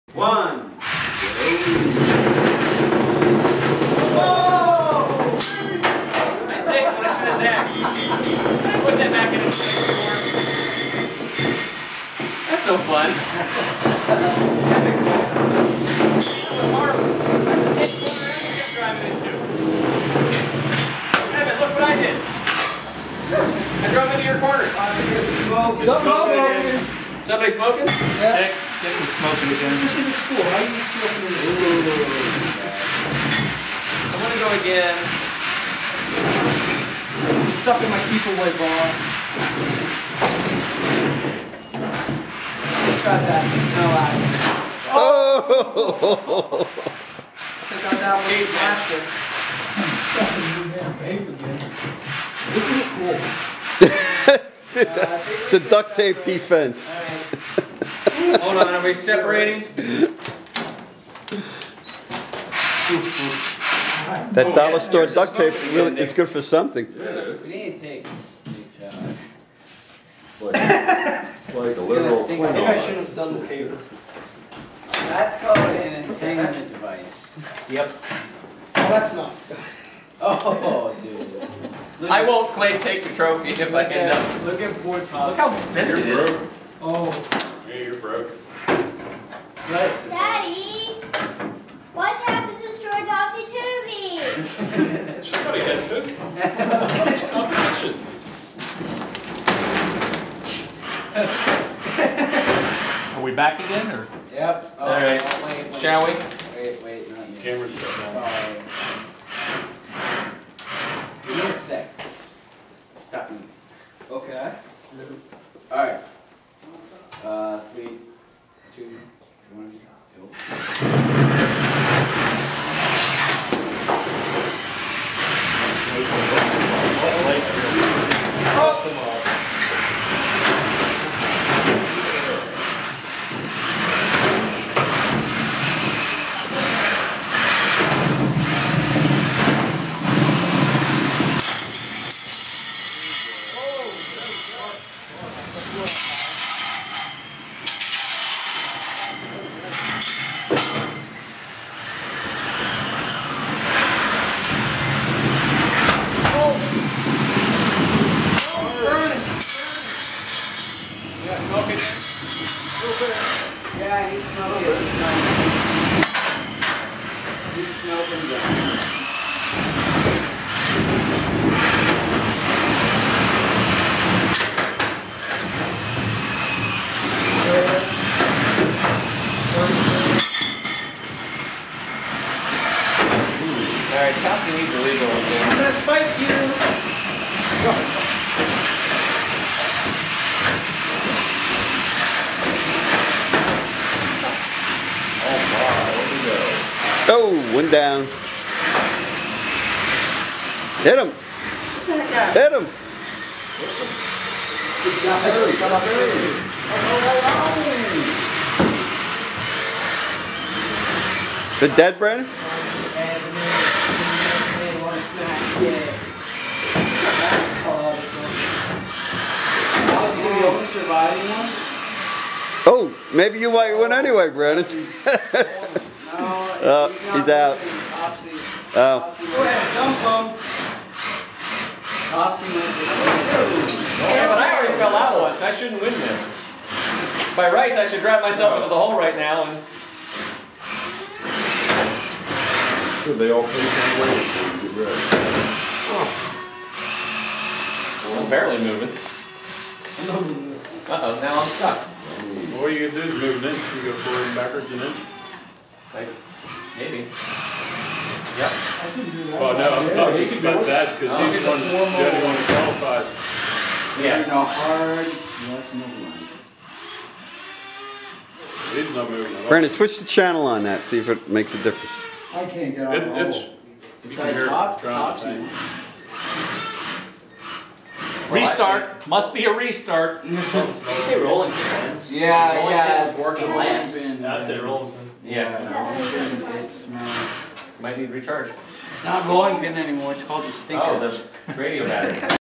Pennbots March 2009 Beetleweight Melee
Naughty duct tape!

Features:
D12
Rolling Pin
The Mole
Topsy Turvey